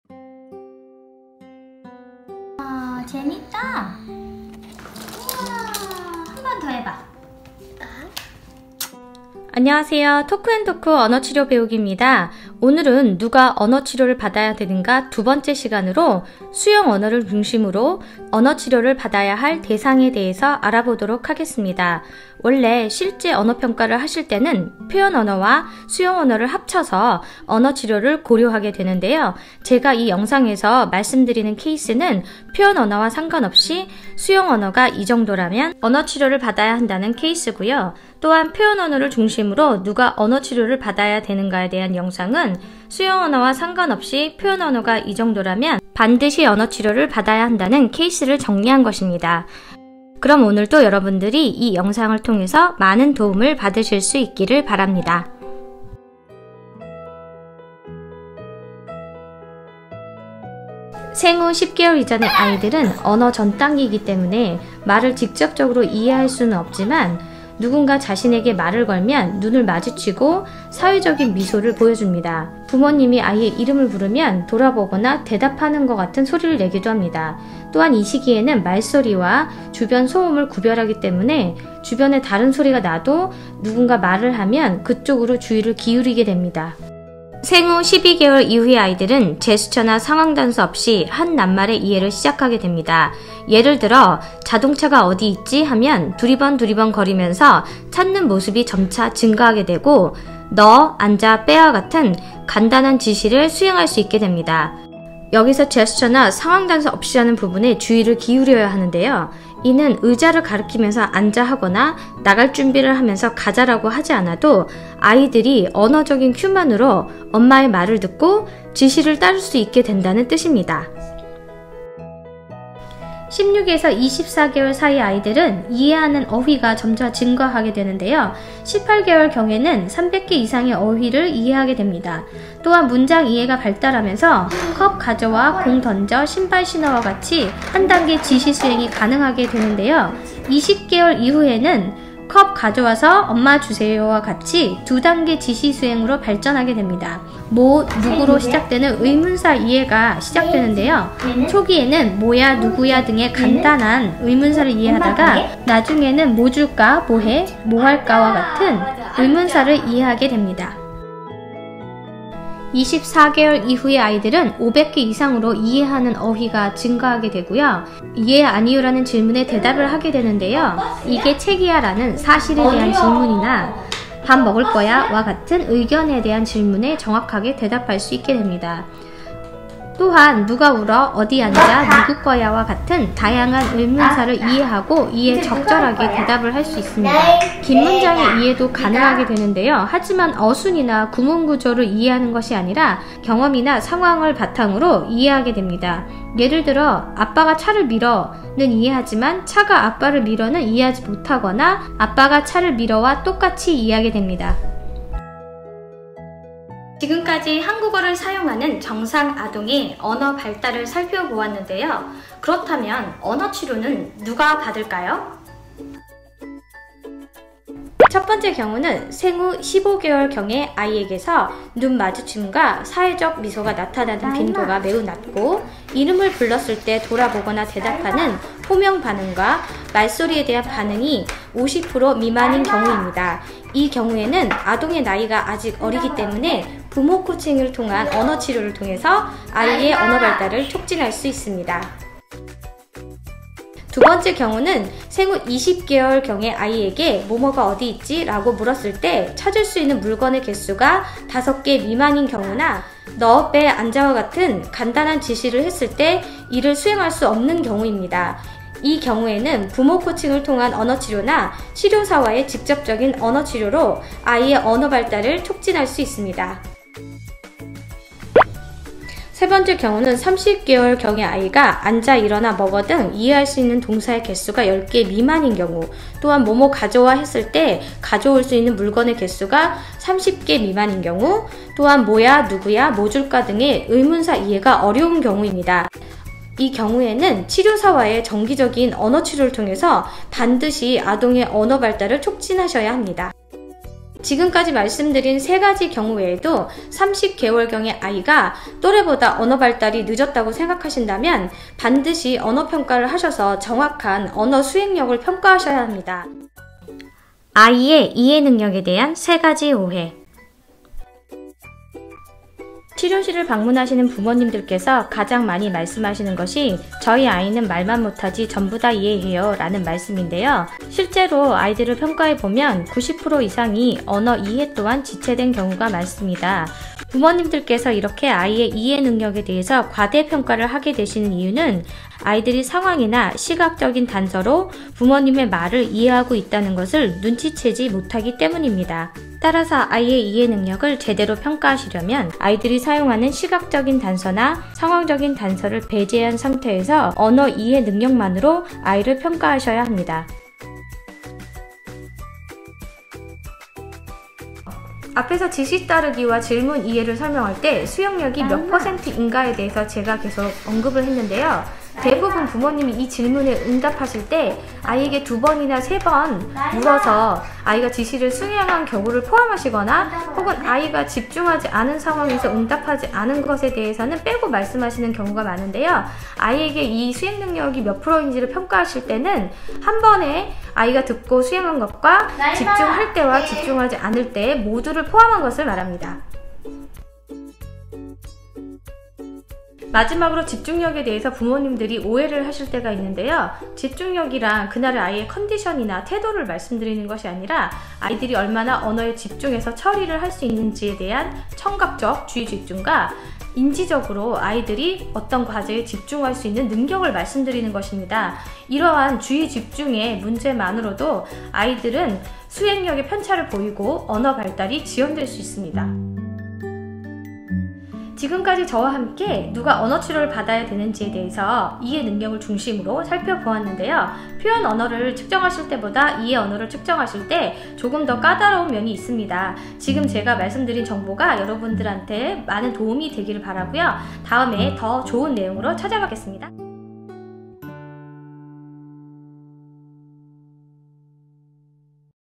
언어치료를 해야하는 세가지 경우-수용언어편_sp03
1:00- 정상언어발달
3:40 언어치료를 해야하는 세가지 경우
5:10 언어이해에 대한 세가지 오해